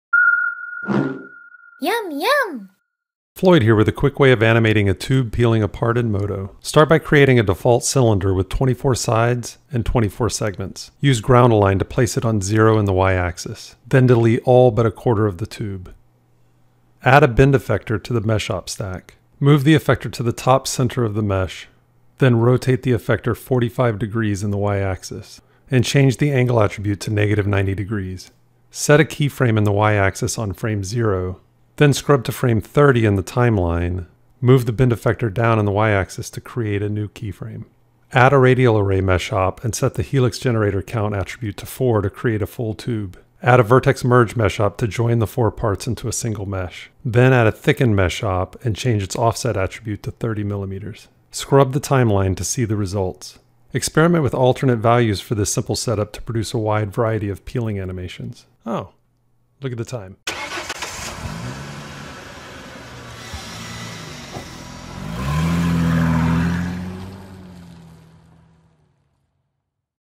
MODO | Animate a Tube Peeling Apart
This video shares a quick way of animating a tube peeling apart in Modo.